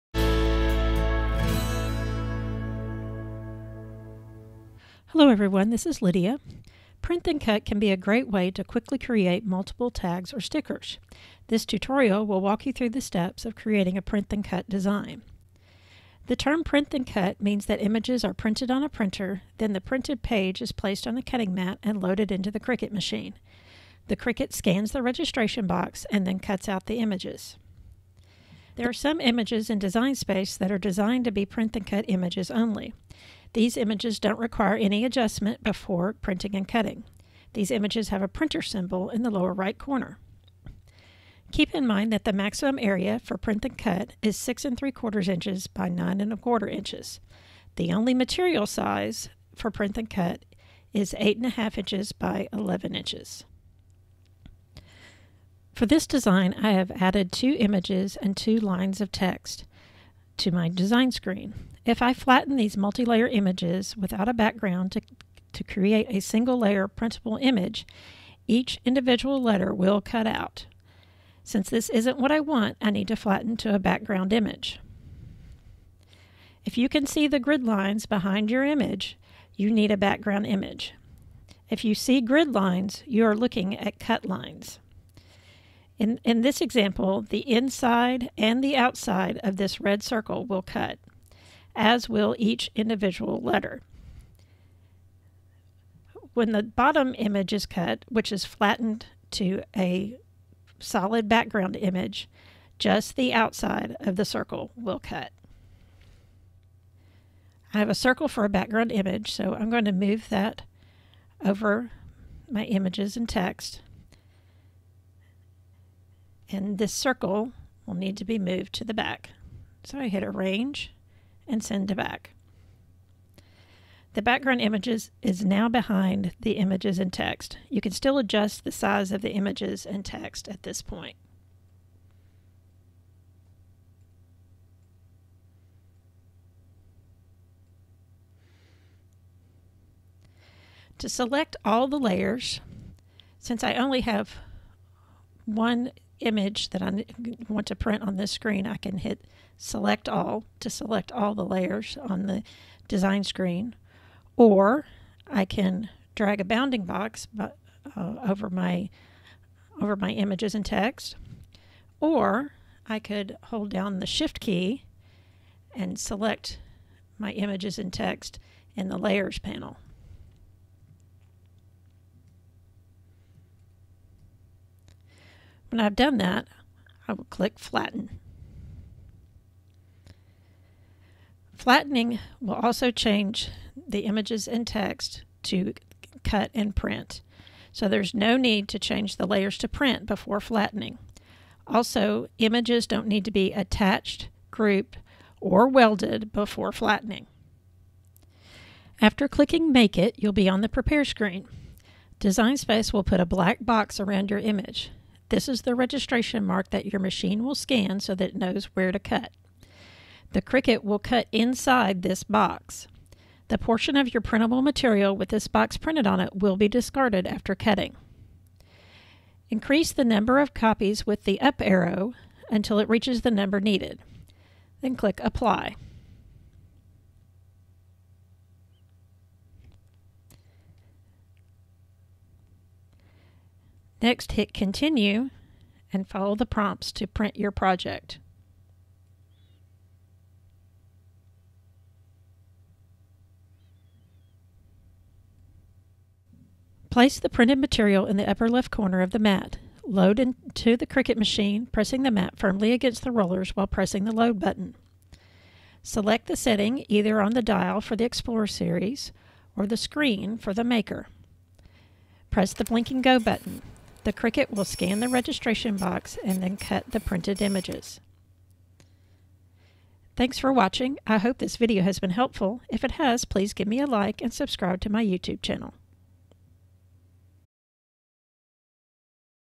Creating a Print then Cut Image in Cricut Design Space
Print then Cut can be a great way to quickly create multiple tags or stickers. This video will walk you through the steps of creating a Print then Cut design.